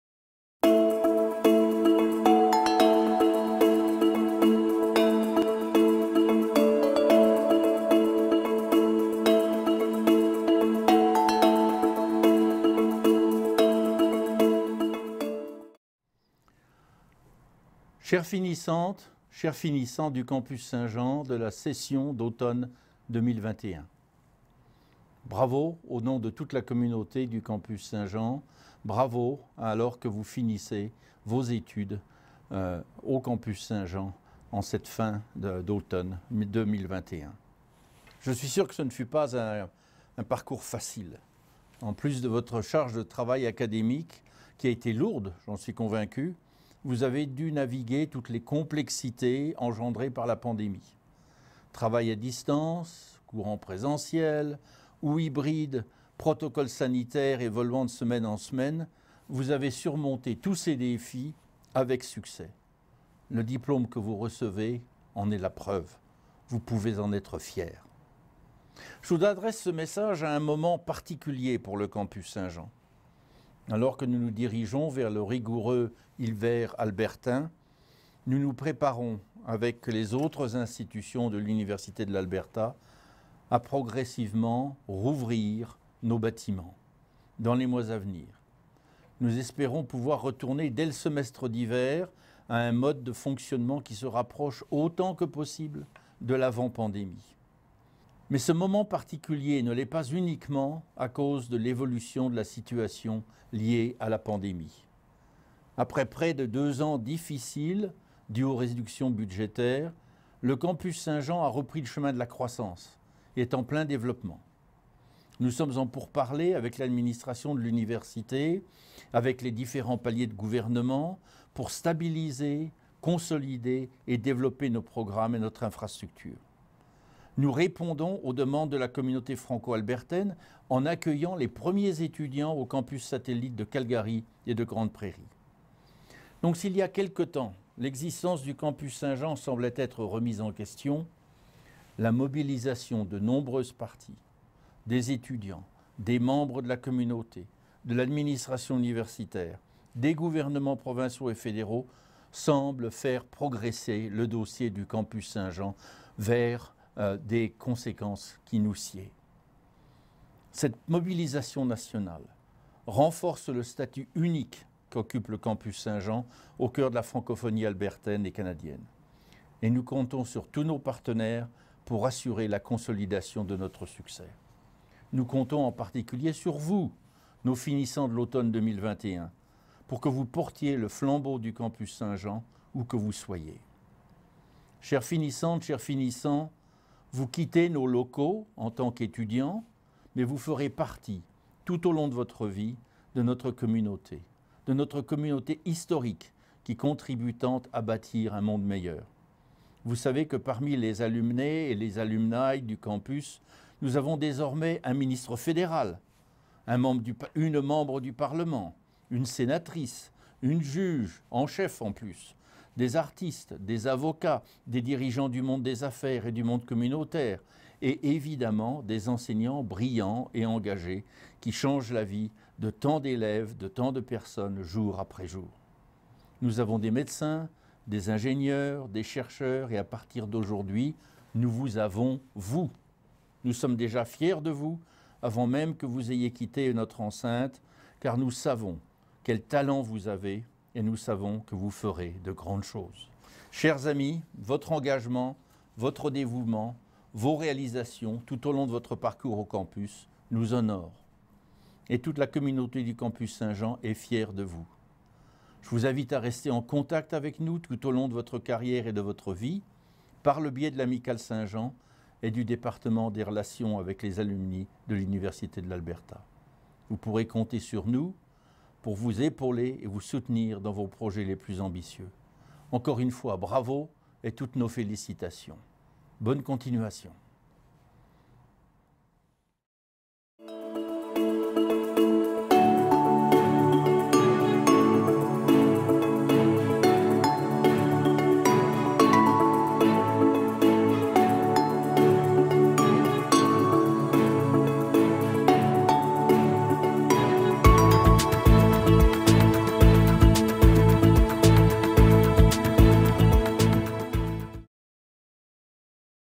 Finissantes et Finissants automne 2021 - Félicitations du Doyen du Campus Saint-Jean
Le Professeur Pierre-Yves Mocquais, Doyen du Campus Saint-Jean, adresse ses félictations aux Finissantes et Finissants de la session d'automne 2021 du Campus Saint-Jean de l'Université de l'Alberta.